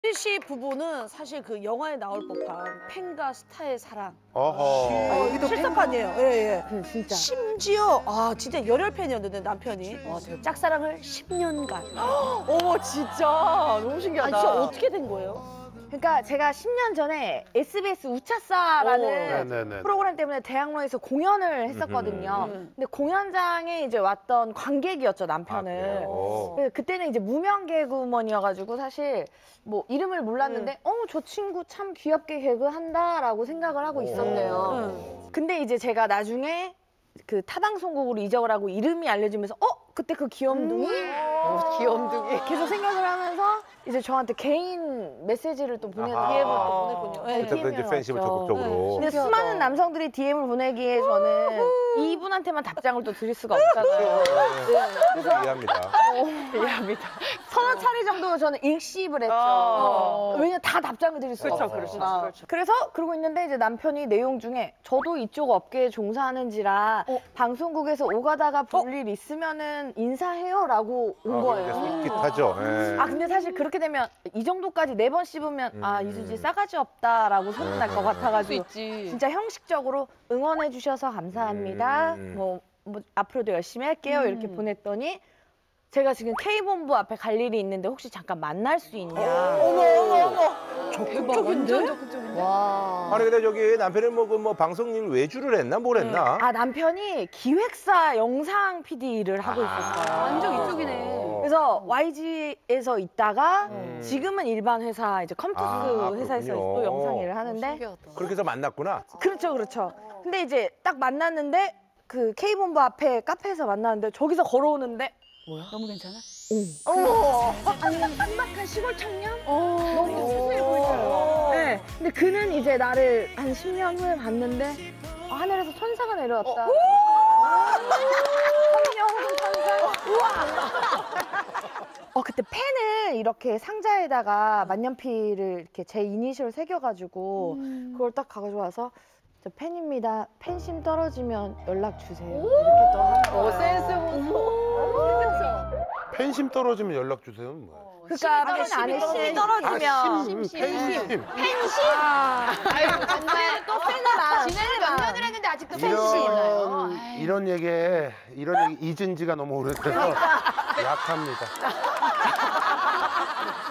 [SUB] ‘팬과 스타의 사랑’ 이수지, 남편과의 운명적인 첫 만남♥
이수지는 남편과의 첫 만남에 대해 이야기한다.